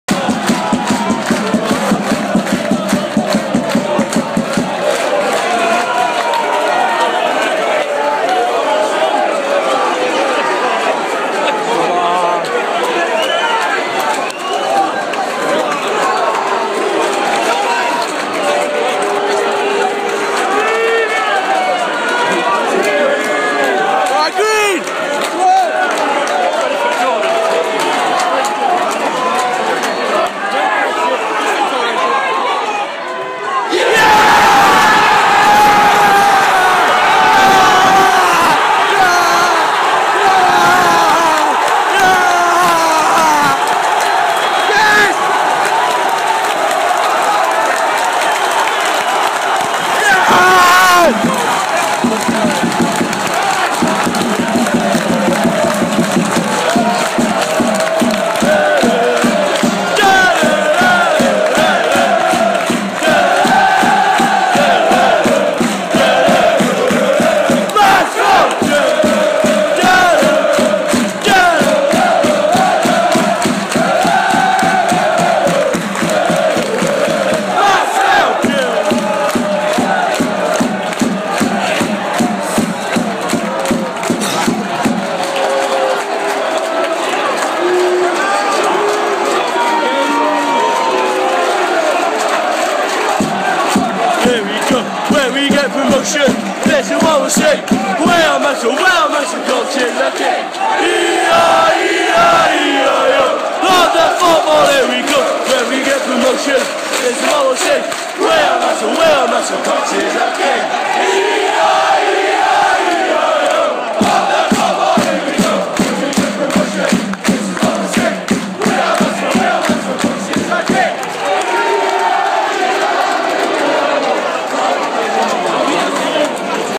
Mansfield town vs Wrexham
Mary greens peno which got us into league 2